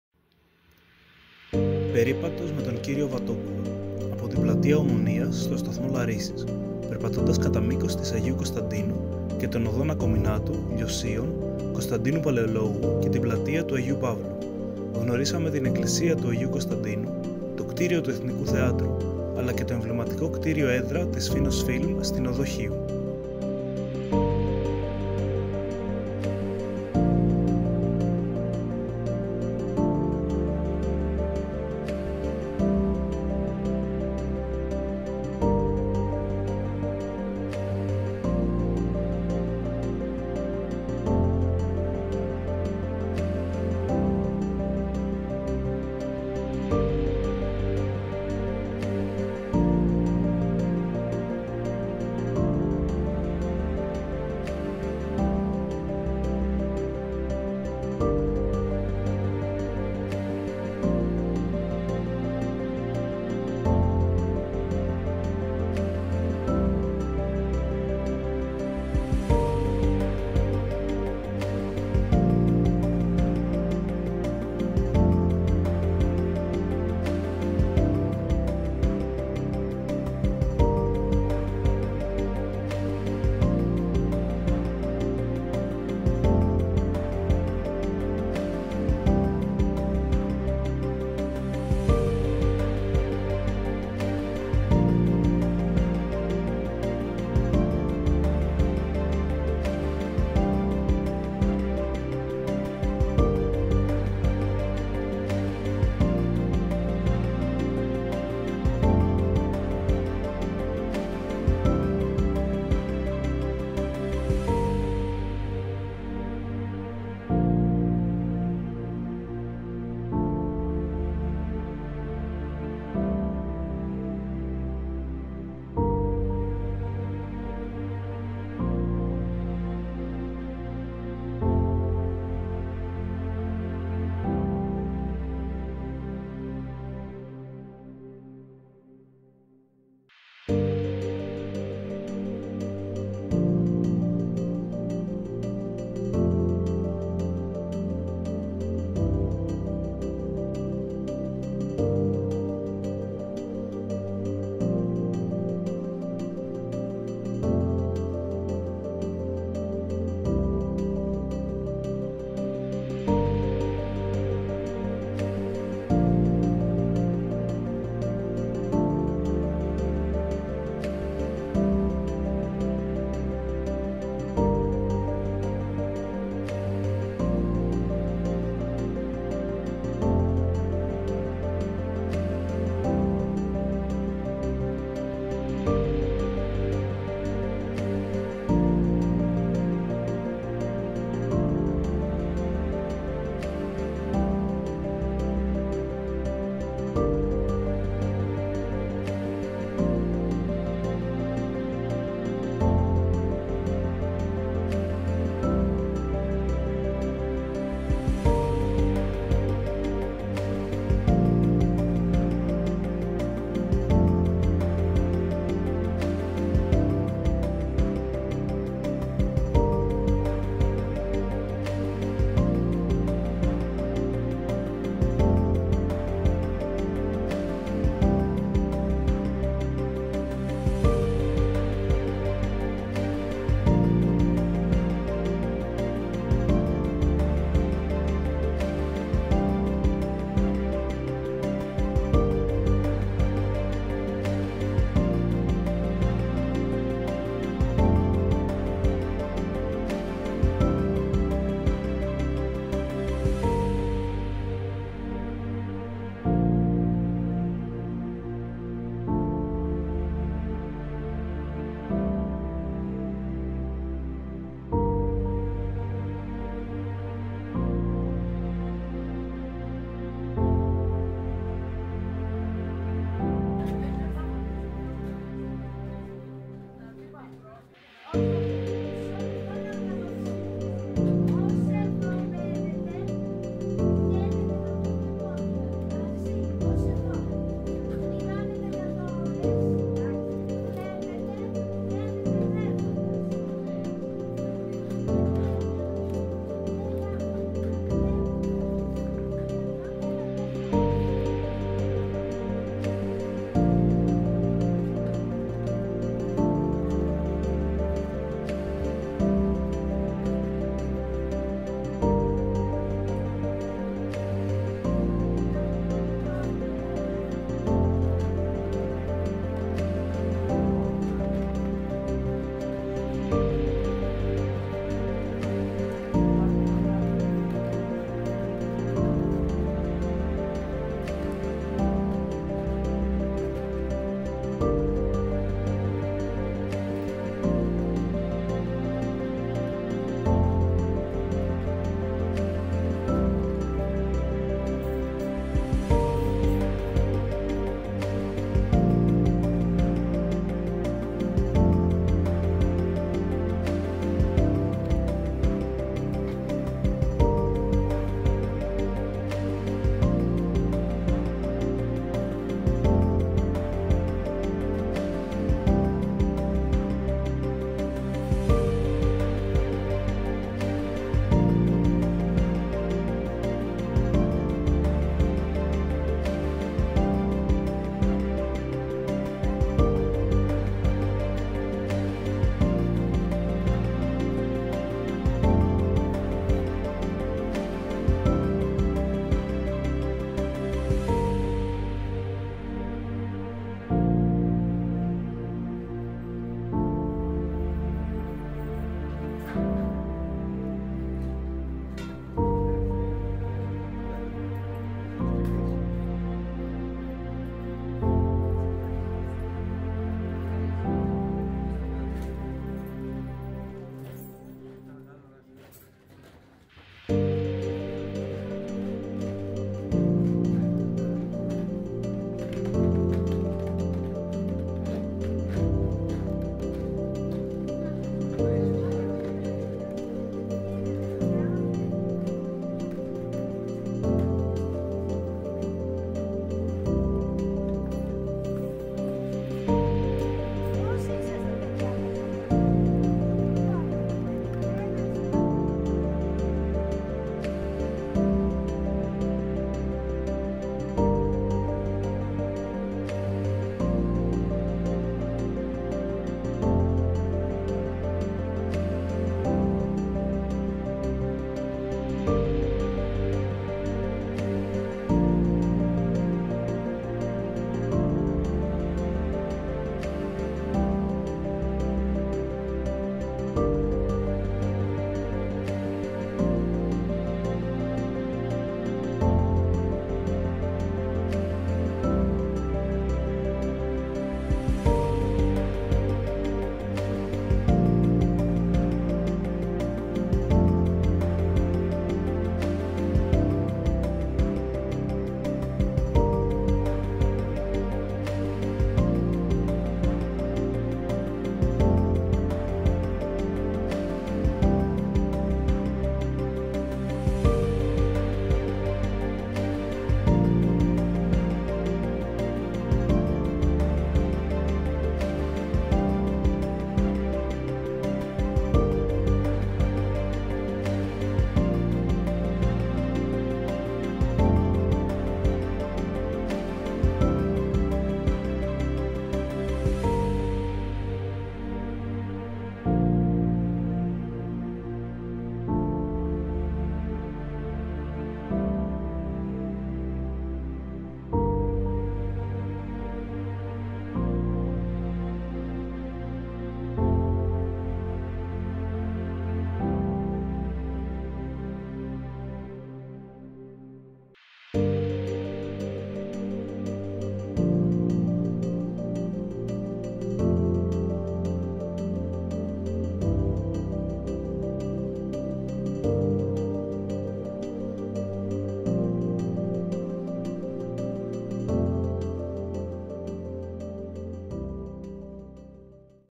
Περίπατος με τον κύριο Βατόπουλο. Από την πλατεία Ομονοίας στον Σταθμό Λαρίσης,
Περίπατος με τον κύριο Βατόπουλο (26/05/2024)
Από την πλατεία Ομονοίας στον Σταθμό Λαρίσης,
περπατώντας κατά μήκος της Αγίου Κωνσταντίνου και
των οδών Ακομινάτου, Λιοσίων, Κωνσταντίνου
Παλαιολόγου και την πλατεία του Αγίου Παύλου.
Γνωρίσαμε την εκκλησία του Αγίου Κωνσταντίνου, το
κτίριο του Εθνικού Θεάτρου, αλλά και το εμβληματικό
κτίριο/έδρα της Finos Film, στην οδό Χίου!
56ο Γυμνάσιο Αθήνας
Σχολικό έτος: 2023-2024